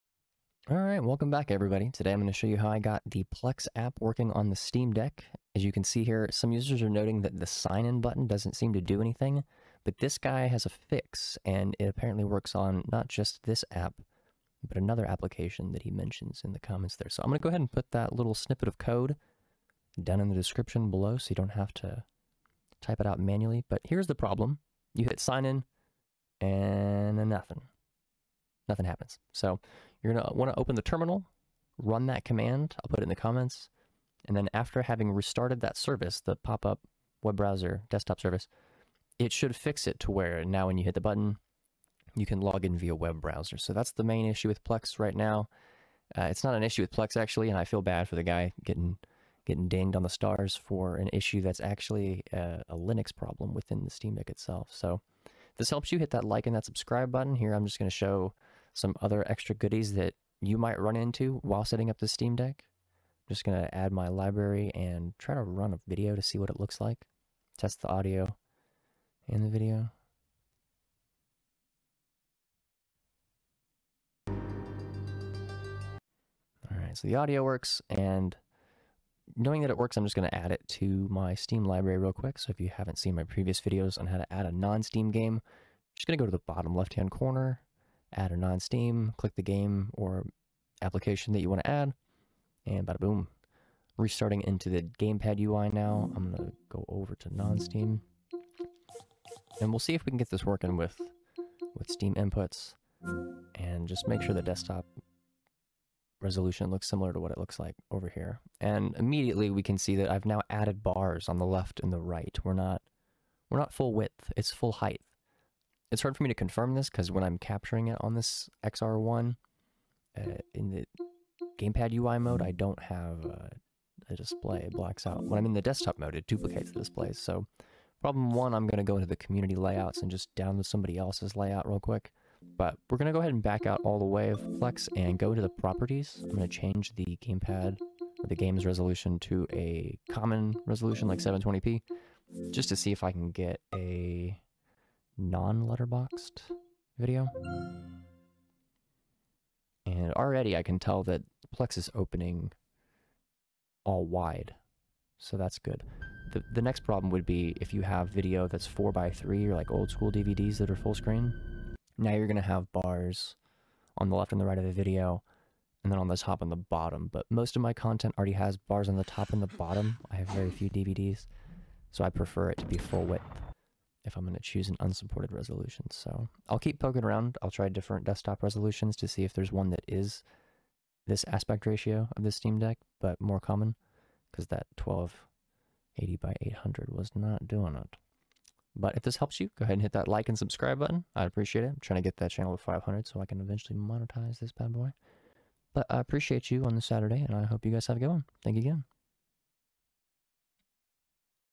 SteamDeck Plex Install - Web Sign In Fix
Users are saying that the Web Sign Up button in Plex and other apps like SDGBoop does nothing. This is a linux problem, not a problem with the app itself, and can be fixed by restarting the service with:

systemctl restart --user xdg-desktop-portal